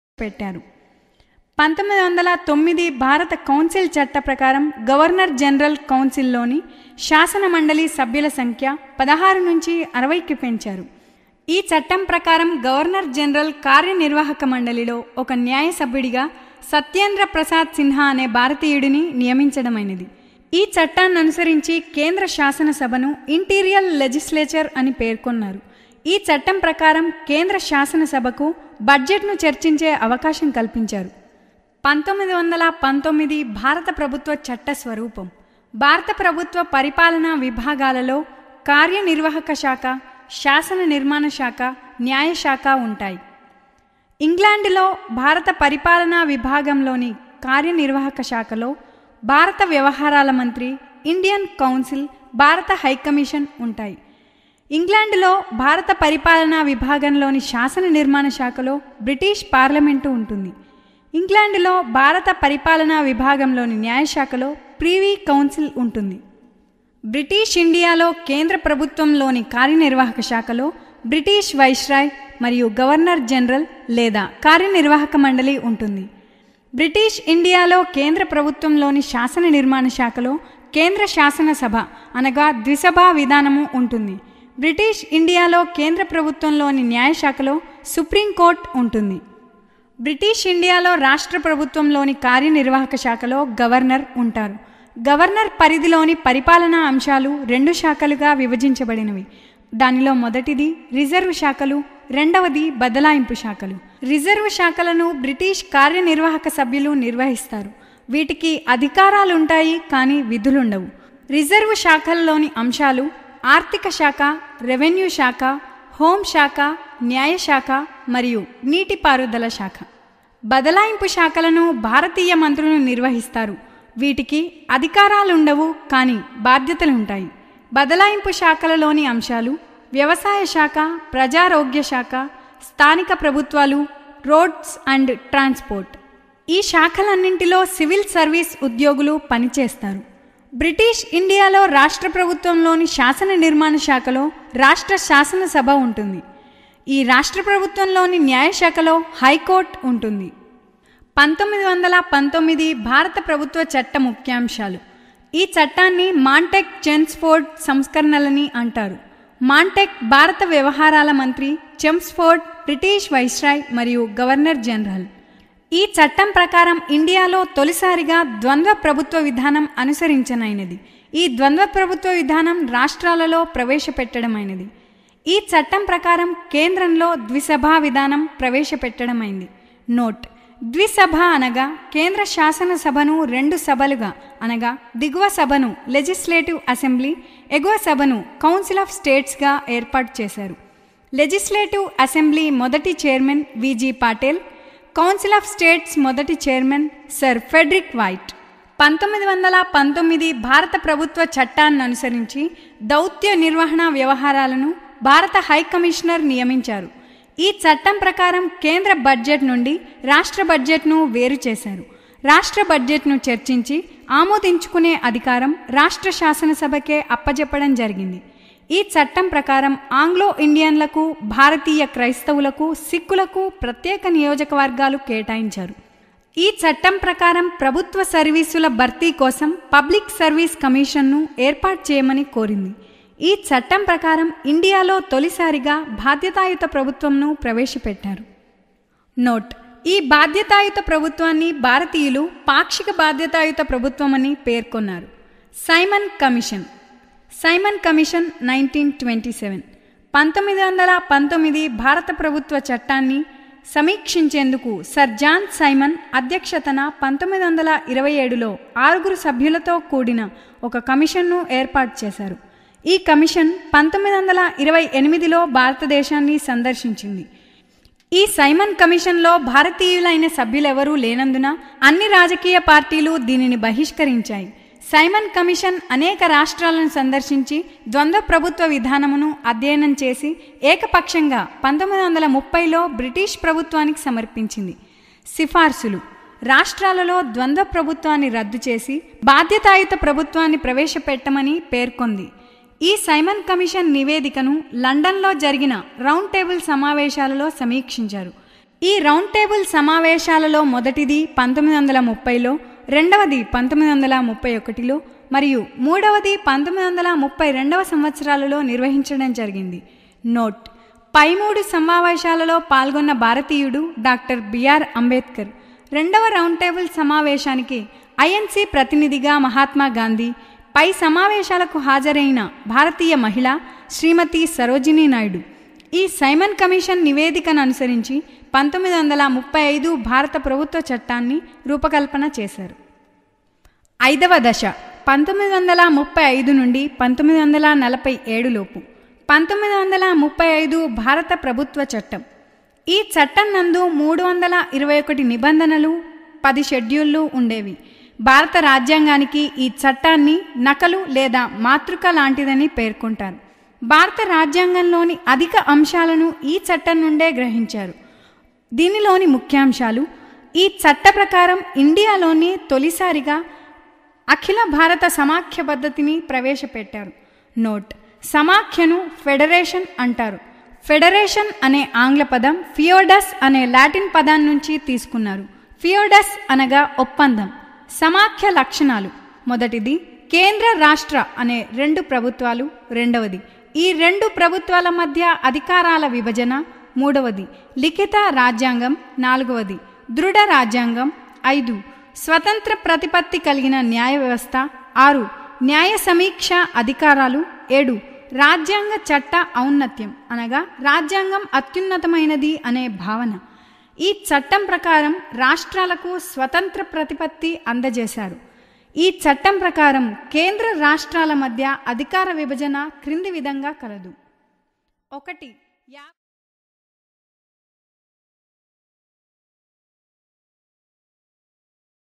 గ్రూప్ 2 | భారత రాజ్యాంగం | Indian Polity - Before Independence Online Class-2
Indian Polity is scouring subject in Group's and Civil's Exams. In this video we explains about the Indian act before independence in British rule.